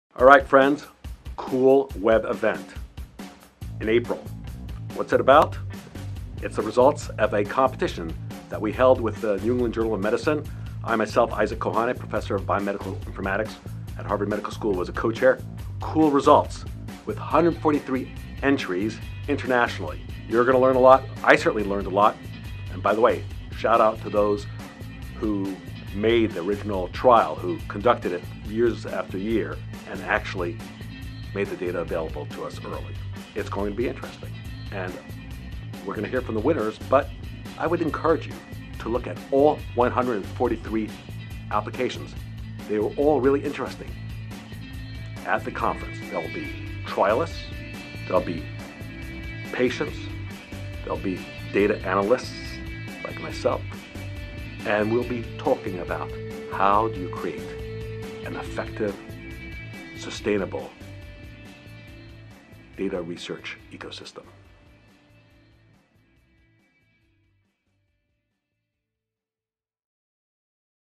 Dr. Isaac Kohane Previews NEJM Data Summit
Join us for "Aligning Incentives for Sharing Clinical Trial Data," a free web event on April 3–4, 2017. Take part in an open conversation among clinical trial stakeholders to help identify sustainable solutions for responsible sharing of data.